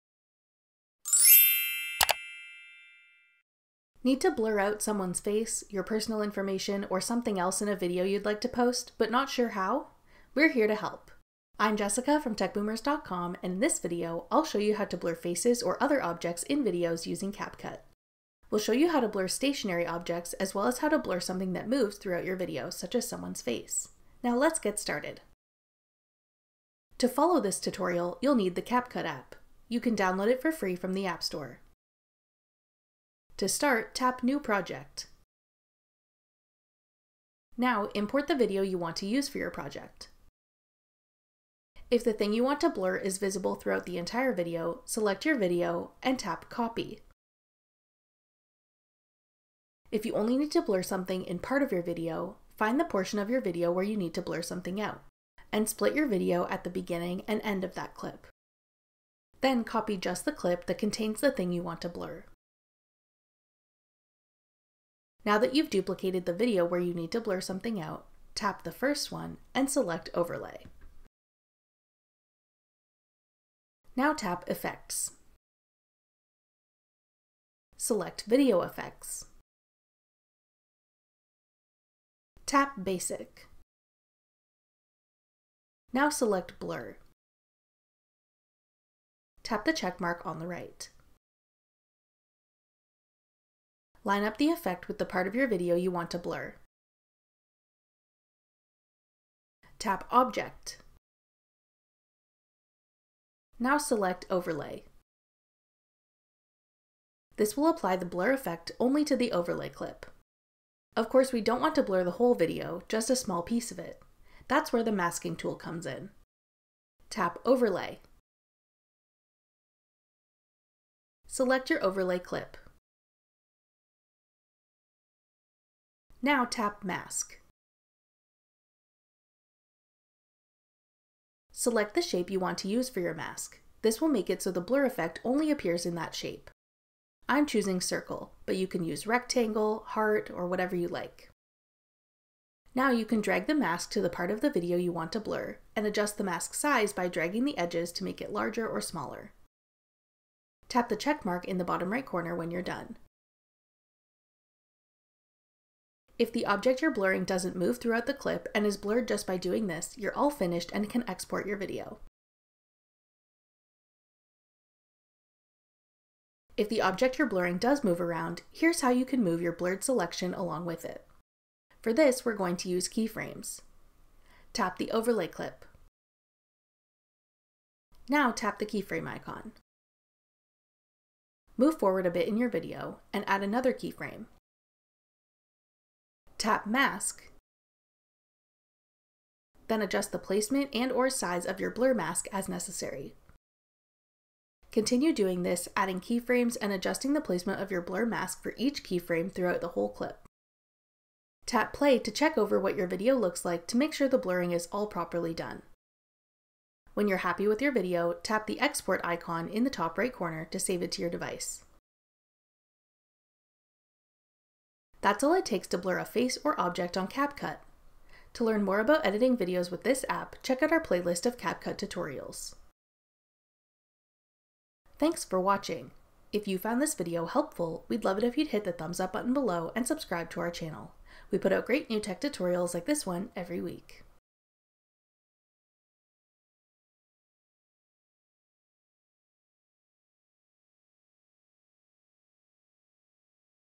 How to Blur Faces or Objects in Videos on CapCut
Need to blur out someone’s face, personal information, or something else in a video you’d like to post, but not sure how? Watch this video to learn how to blur faces and objects in videos using CapCut!

To follow this tutorial, you’ll need the CapCut app. You can download it for free from the app store. To start, tap New Project. Now, import the video you want to use for your project. If the thing you want to blur is visible throughout the entire video, select your video and tap Copy. If you only need to blur something in part of your video, find the portion of the video where you need to blur something out, and split your video at the beginning and end of that clip. Then, copy just the clip that contains the thing you want to blur. Now that you’ve duplicated the video where you need to blur something out, tap the first one, then select Overlay. Now, tap Effects. Select Video Effects. Tap Basic. Now select Blur. Tap the check mark on the right. Line up the effect with the part of your video you want to blur. Tap Object, now select Overlay. This will apply the blur effect only to the overlay clip.

Of course we don’t want to blur the whole video, just a small piece of it. That’s where the masking tool comes in. Tap Overlay. Tap your overlay clip. Now, tap Mask. Select the shape you want to use for your mask. This will make it so the blur effect only appears in that shape. I’m choosing circle. Now, you can drag the mask to the part of the video you want to blur, and adjust the mask size by dragging the edges to make it larger or smaller. Tap the check mark in the bottom right corner when you’re done. If the object you’re blurring doesn’t move throughout the clip, and is blurred just by doing this, you’re all finished and can export your video.

If the object you’re blurring does move around, here’s how you can move your blurred selection along with it. For this we’re going to use Keyframes. Tap the overlay clip. Now tap the Keyframe icon. Move forward a bit in your video and add another keyframe. Tap Mask, then adjust the placement and/or size of your blur mask as necessary. Continue doing this and adding keyframes and adjusting the placement of your blur mask for each keyframe throughout your whole clip. Tap Play to check over what your video looks like to make sure the blurring is all properly done. When you’re happy with your video, tap the Export icon in the top right corner to save it to your device.

That’s all it takes to blur a face or object on CapCut.

Chapters:
0:00 Intro
0:27 How to Blur Objects or Faces on CapCut
2:46 How to Blur Moving Objects or Faces on CapCut
3:35 Wrap Up